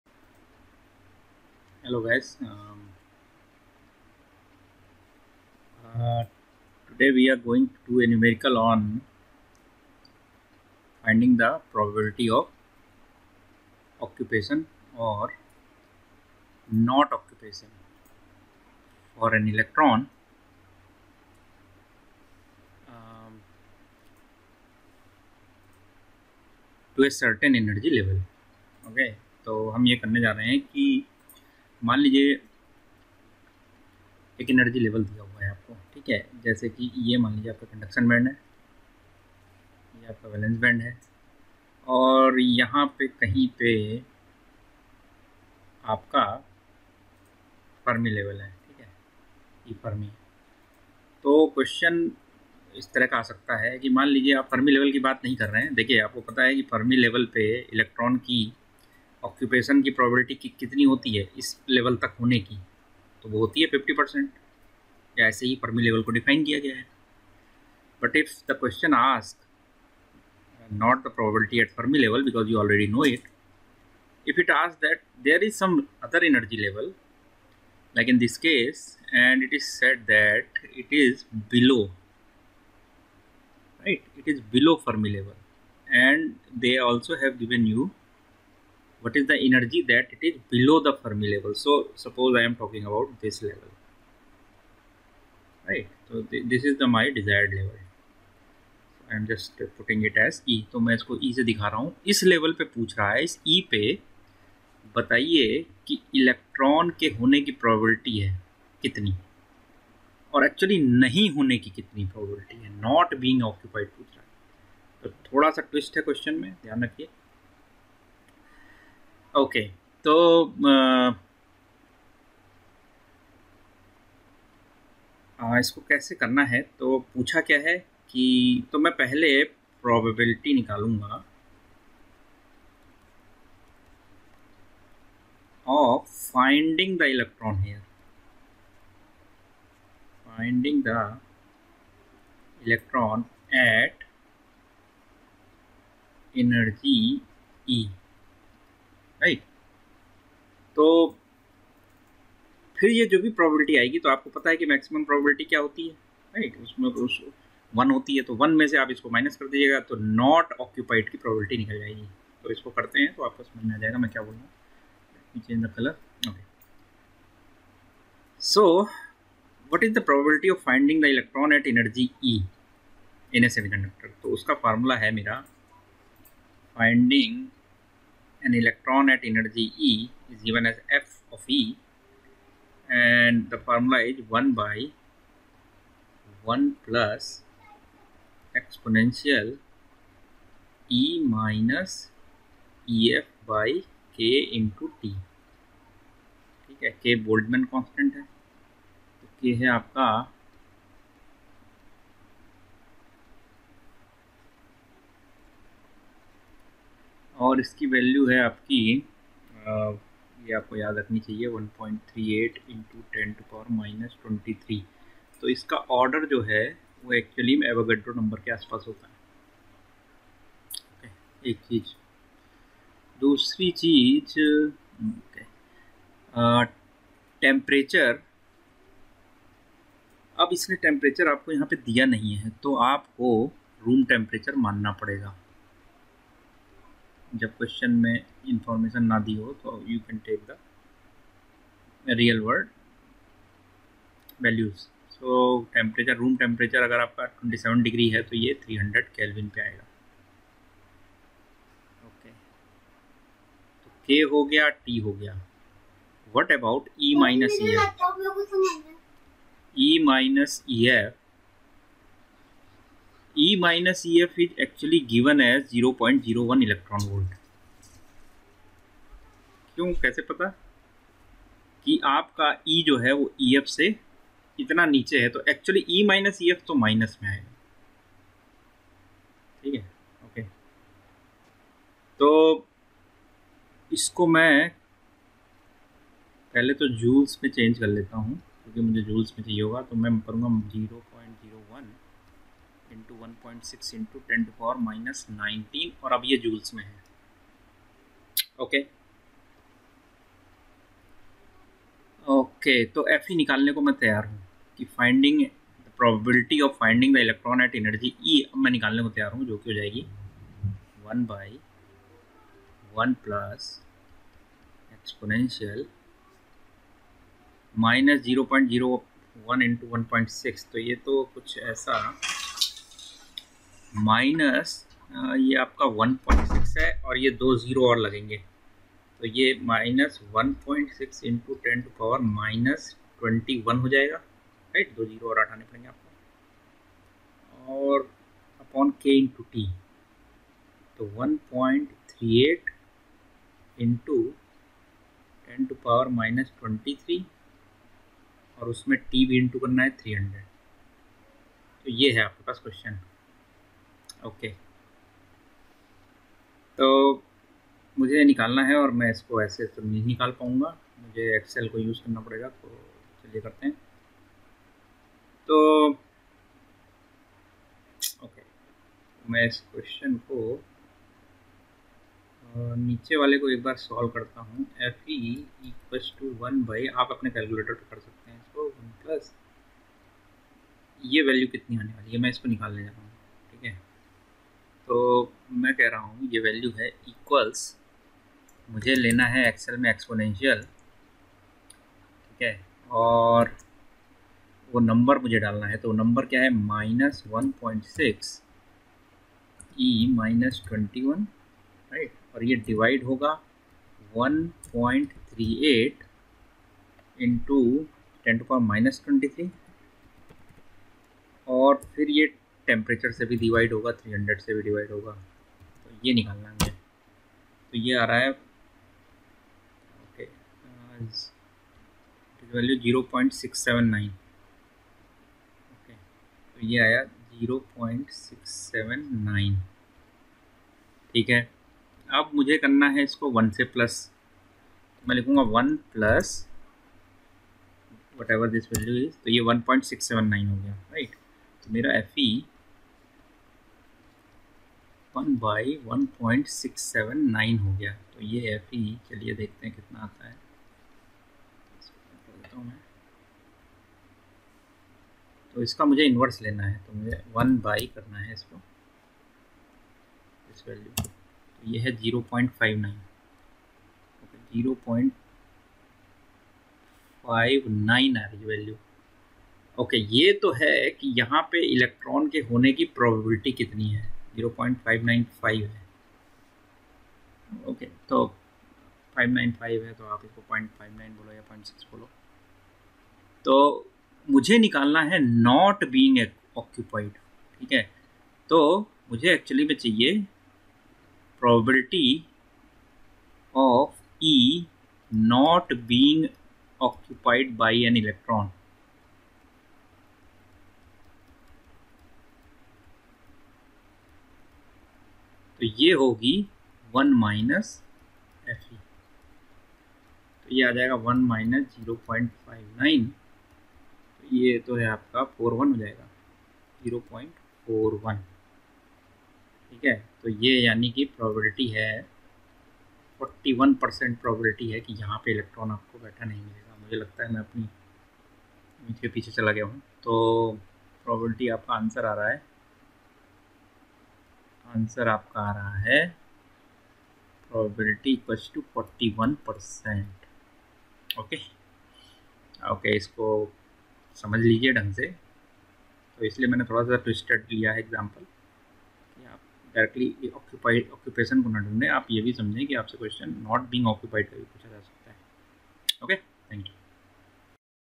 Numerical on Fermi Level of Semiconductors 01
Finding the Probability of NON Occupation of Energy Level

Language English/Hindi

Dr Gagan